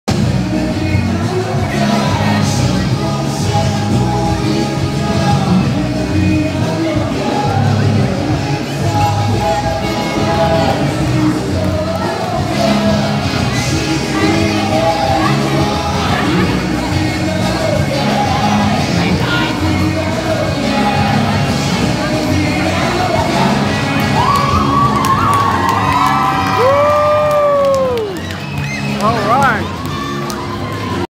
Shawn & Sue's Flash Mob
Flash Mob at Shawn & Sue's Heart Full of Hope Radiothon to benefit the Children's Specialized Hospital. Live at the Ocean County Mall! 92.7 WOBM